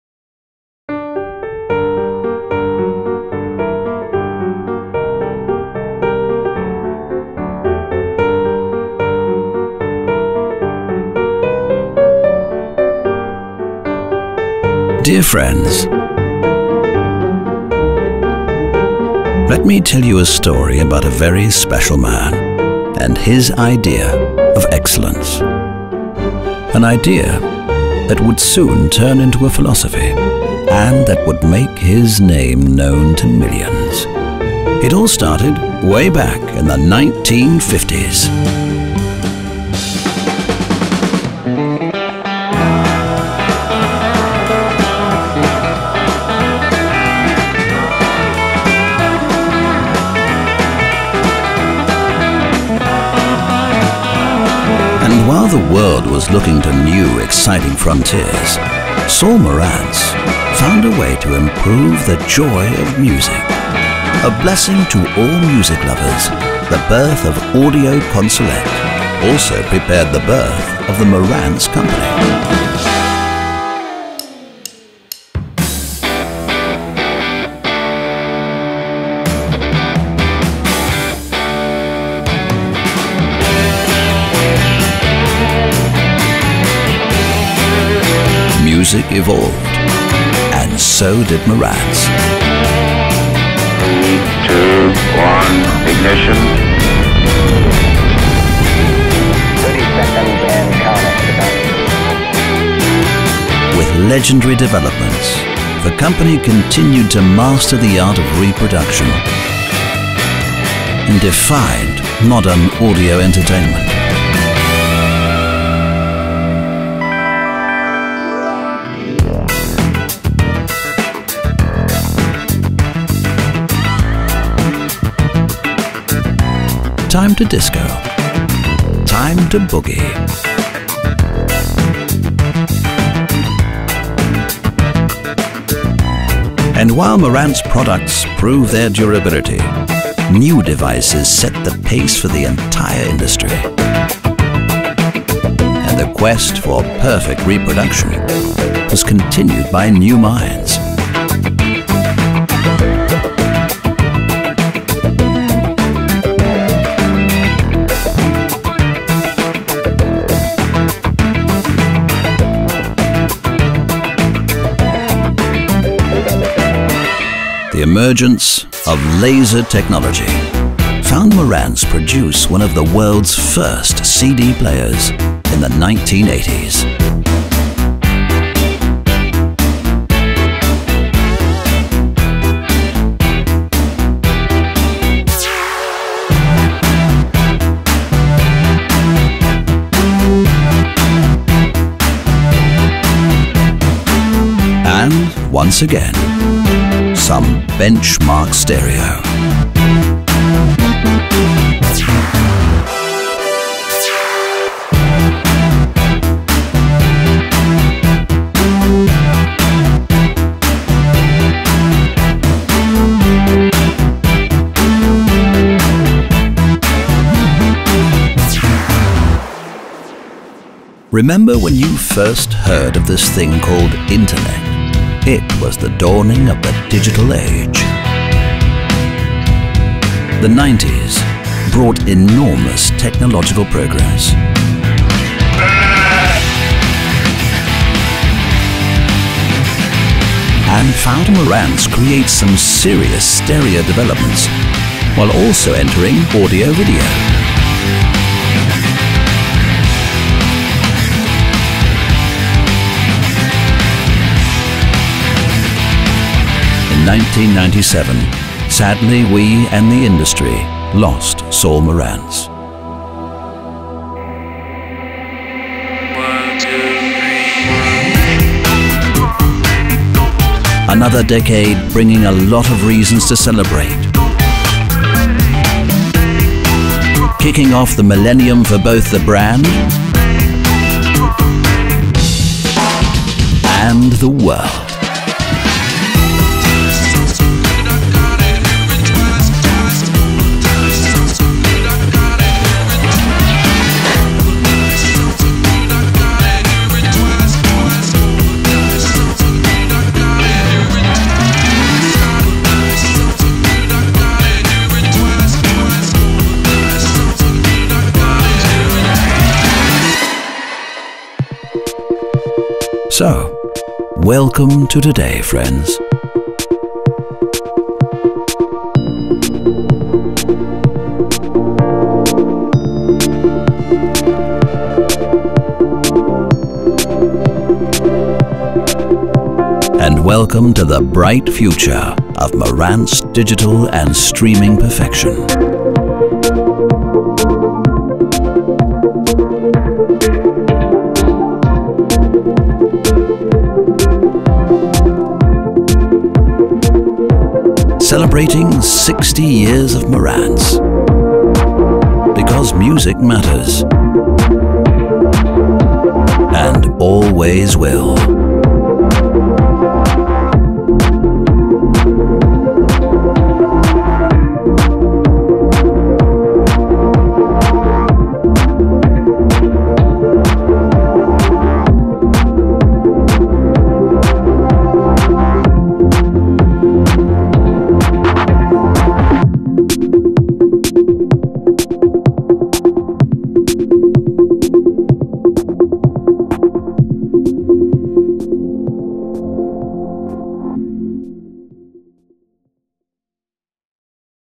Marantz 60 Year Anniversary Video : A Tribute to Music
More than any other person, Saul Marantz defined premium home entertainment. Driven by his passion for music and his accomplishments as a classical guitarist -- accomplishments that led to a close friendship with Andres Segovia -- he was never satisfied with the "hi fi" equipment of his day. So he built better; first in his basement, later in a factory. His talent for industrial design and his ability to infuse talented engineers like Sidney Smith and others with his vision resulted in legendary products. The Model 7 preamplifier. The Model 8 and, soon after, the 8B power amplifier. And insured that his company would remain a premiere name in the industry he helped establish.